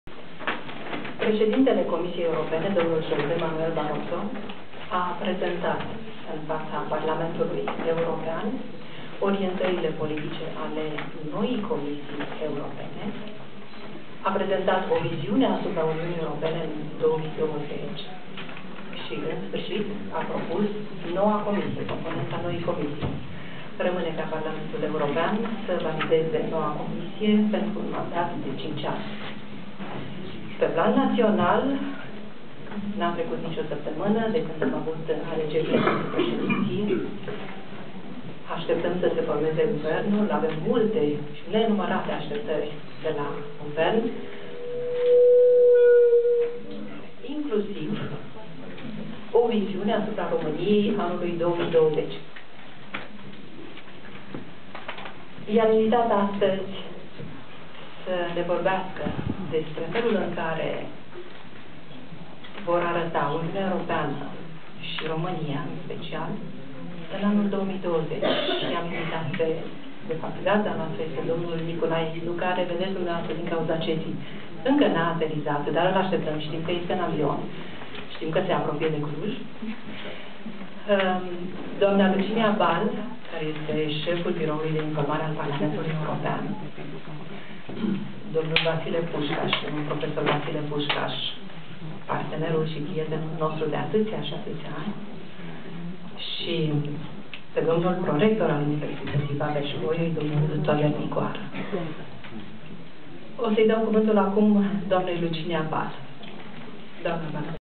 Cum vor arăta Uniunea Europeană şi România în anul 2020?
UE 2020 - România 2020 - deschiderea oficială şi dezbaterea din cadrul conferinţei Centrelor de informare europeană - Cluj, 10 decembrie 2009. Moderator, doamna Doina Şerban, Reprezentanţa CE în România.